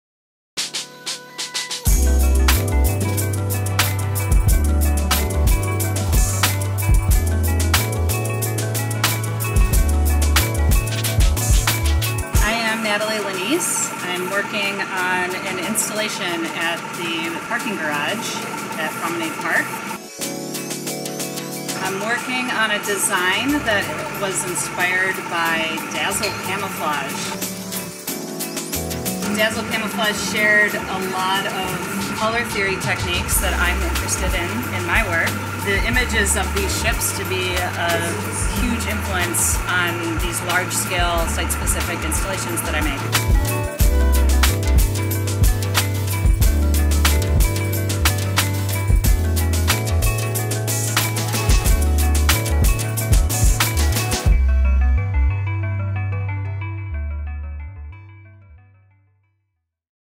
The Dazzle Colonnade by Natalie Lanese
The Dazzle Colonnade is located on ProMedica's Depot Garage. Funded by ProMedica, administered by The Arts Commission. Video by Nick Corbin.